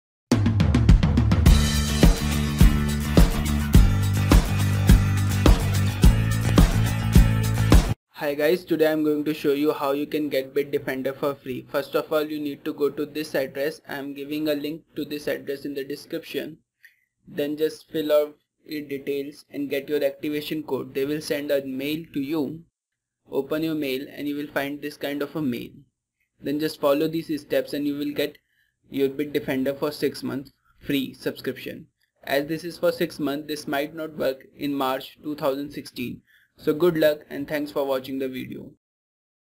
Bitdefender Total Internet Security 2016 for free - best anti virus
Bitdefender is one of the best anti virus software for your computer and phone it give a complete solution it does not only secure computer from virus but also give internet security from malaware.

In this video, I 'am going to show you how to download antivirus  and get its pro account for free BITDEFENDER'S 6 MONTHS FREE SUBSCRIPTION, OFFICIALLY.